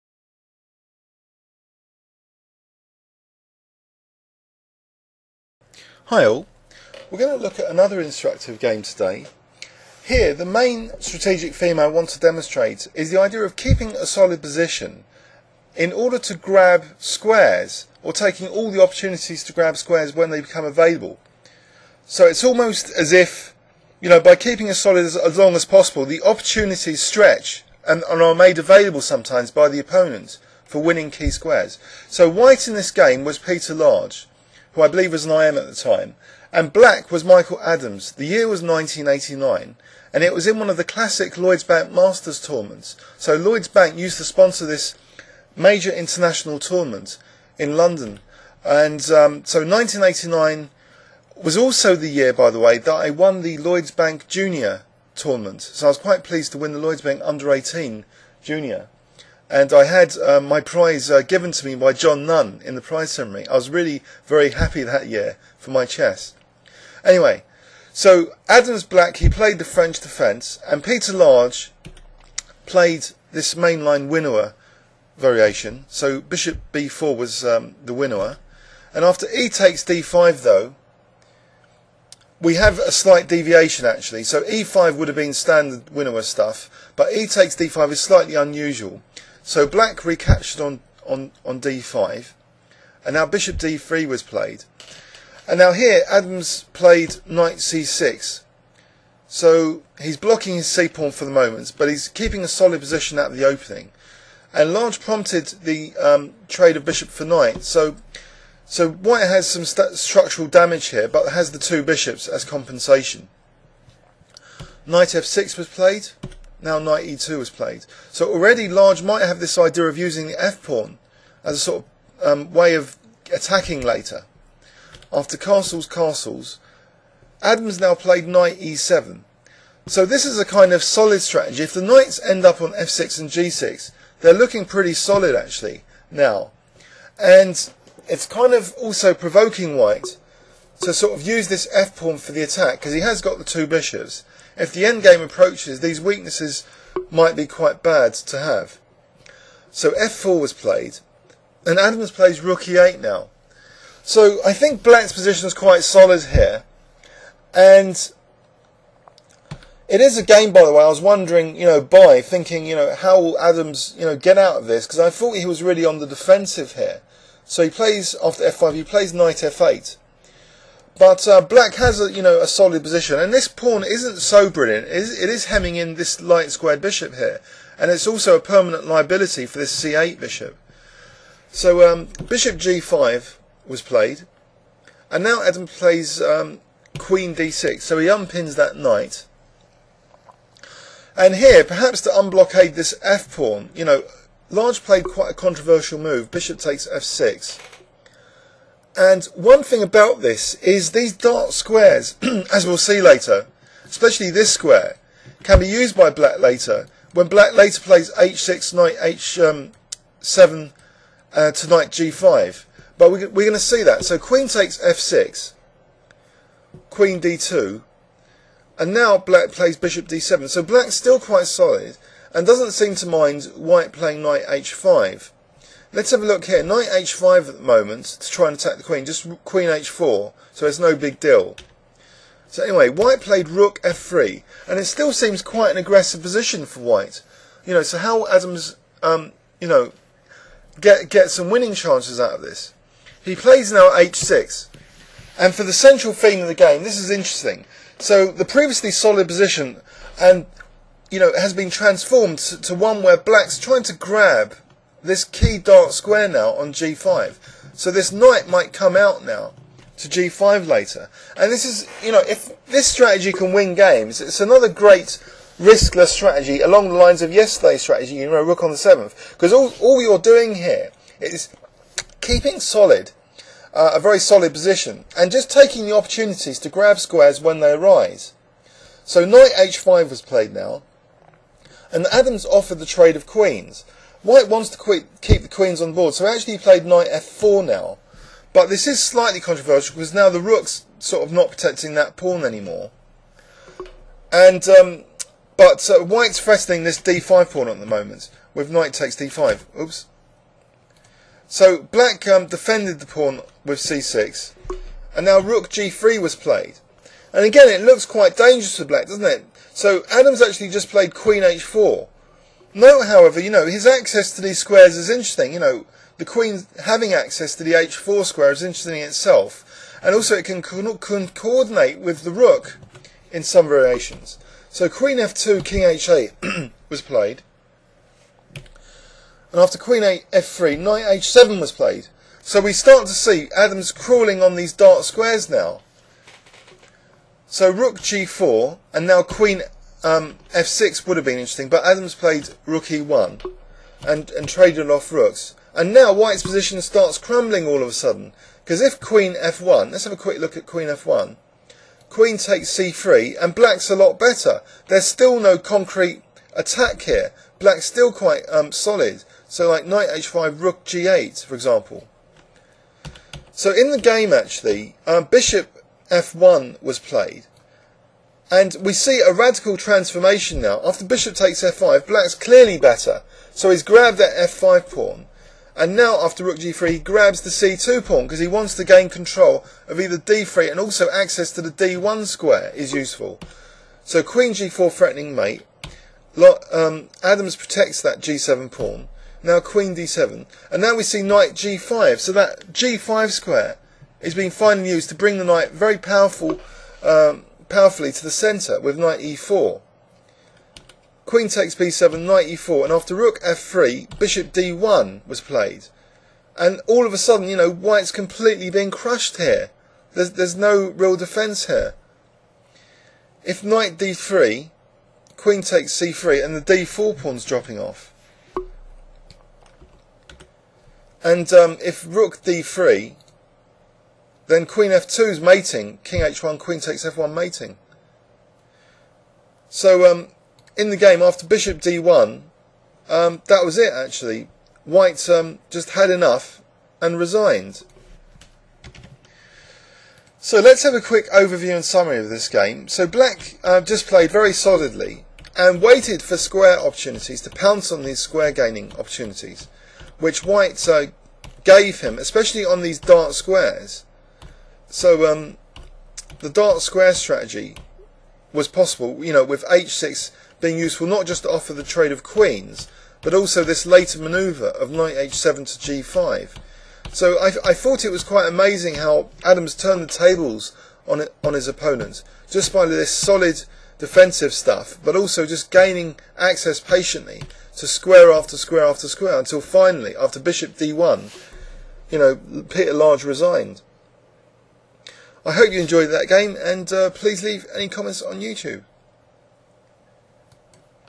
French Defence Delayed Exchange: Peter Large vs Michael Adams - Grabbing squares (Chessworld.net)
Grabbing squares - IM Peter Large vs GM Michael Adams

[Event "London Lloyds"]
[Site "London Lloyds"]
[Date "1989.??.??"]
[EventDate "?"]
[Round "?"]
[Result "0-1"]
[White "Peter G Large"]
[Black "Michael Adams"]
[ECO "C01"]
[WhiteElo "?"]
[BlackElo "?"]
[PlyCount "58"]

1.e4 e6 2.d4 d5 3.Nc3 Bb4 4.exd5 exd5 5.Bd3 Nc6 6.a3 Bxc3+
7.bxc3 Nf6 8.Ne2 O-O 9.O-O Ne7 10.Ng3 Ng6 11.f4 Re8 12.f5 Nf8
13.Bg5 Qd6 14.Bxf6 Qxf6 15.Qd2 Bd7 16.Rf3 h6 17.Nh5 Qg5 18.Nf4
c6 19.Rg3 Qh4 20.Qf2 Kh8 21.Qf3 Nh7 22.Rg4 Re1+ 23.Rxe1 Qxe1+
24.Bf1 Bxf5 25.Rg3 Bxc2 26.Qg4 Rg8 27.Qd7 Ng5 28.Qxb7 Ne4